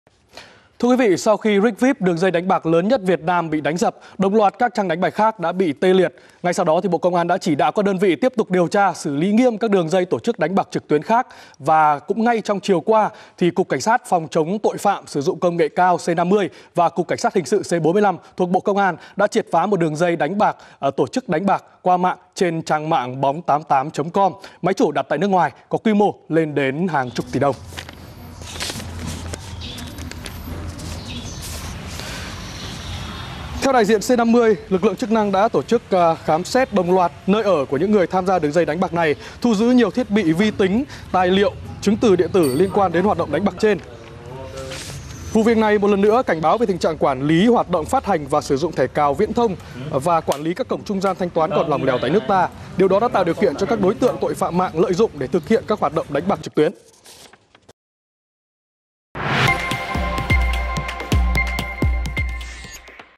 Triệt phá đường dây đánh bạc trên trang mạng bong88.com - Tin Tức VTV24
Đường dây đánh bạc trên trang mạng bong88.com

➡ Tin tức: goo.gl/ysuazt
➡ Tài Chính: goo.gl/rbyNAs